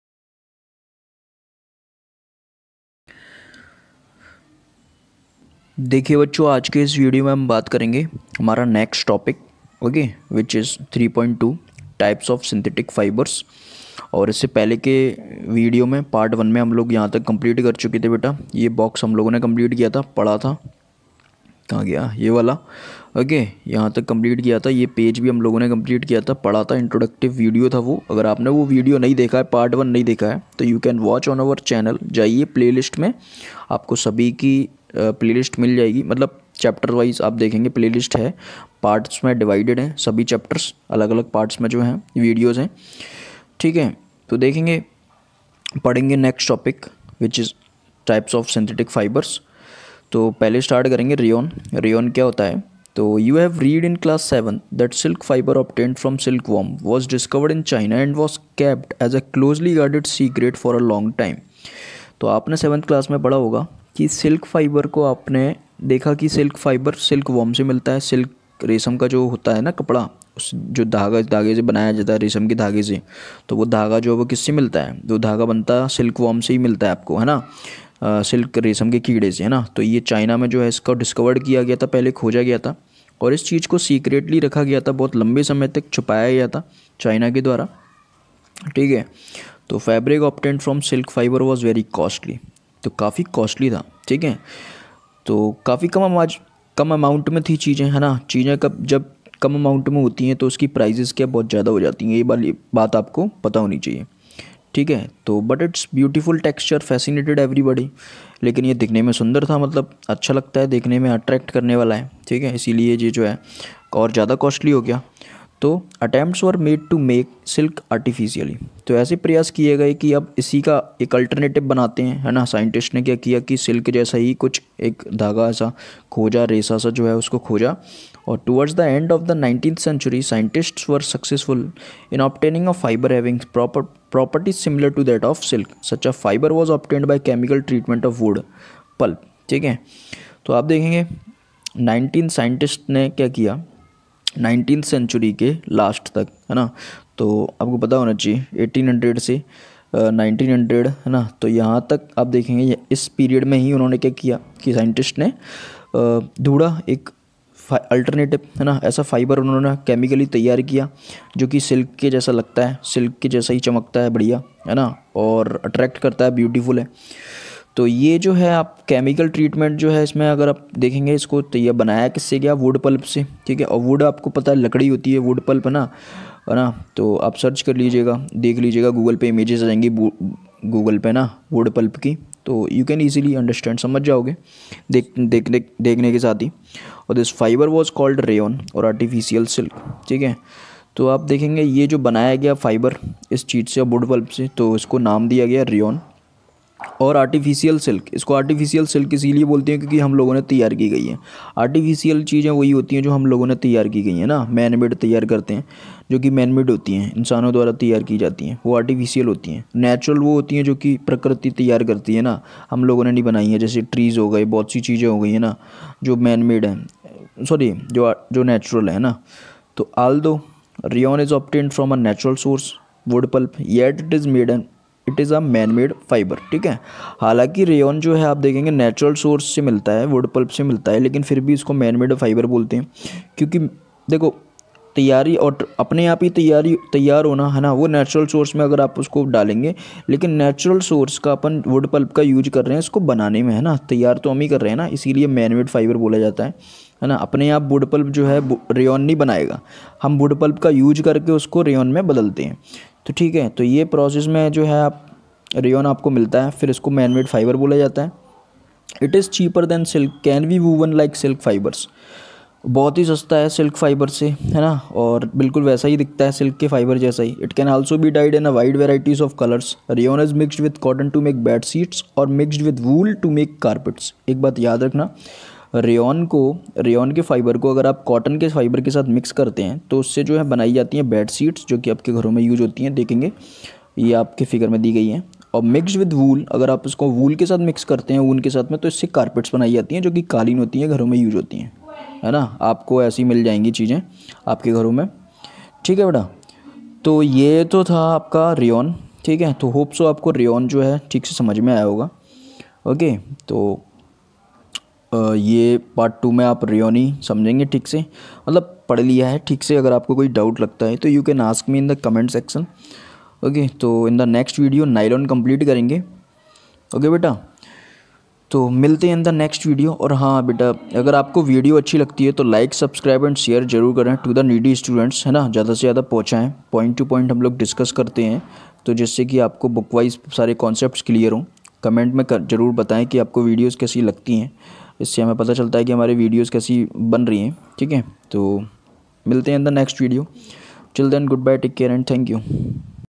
Synthetic fibres and plastics | Rayon | class 8 | science | ch-3(Part-2) | NCERT | In Hindi
Ncert's Books summary 📚📖
Point to point Book explanation✏️
Bookwise concepts and learning 📝